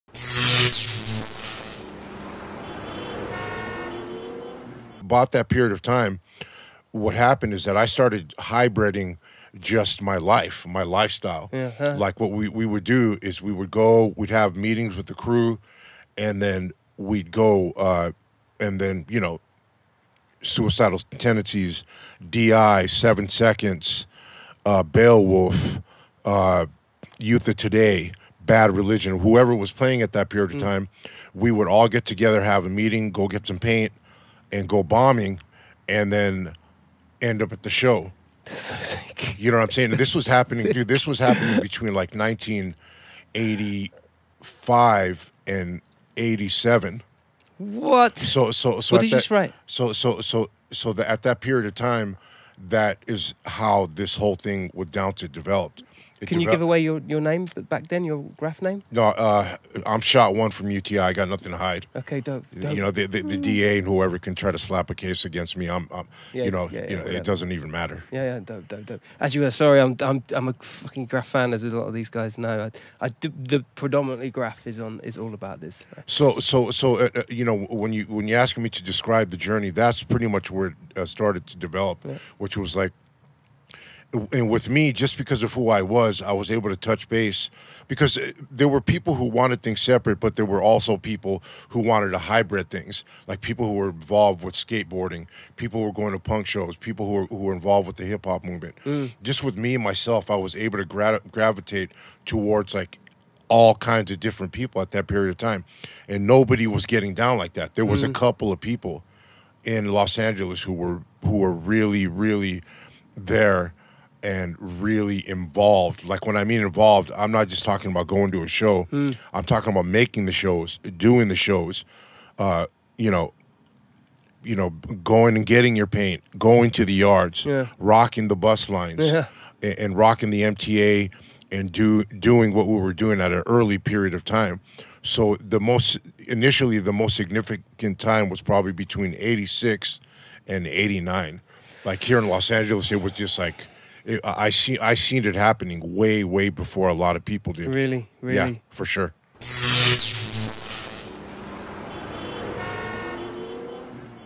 DOWNSET TALKS 1980s LA STREET CULTURE
Killa Kela sits down with Ray and Roy from DOWNSET. Deeply influenced by the Los Angeles Punk, Hip-hop and graffiti scenes of the 1980s, they give us insight into exactly what is was like back then.

This clip is from the KKPC#149 Punk Graffiti and Rap, this is DOWNSET from Los Angeles!

Follow Killa Kela on all his socials.

KILLA KELA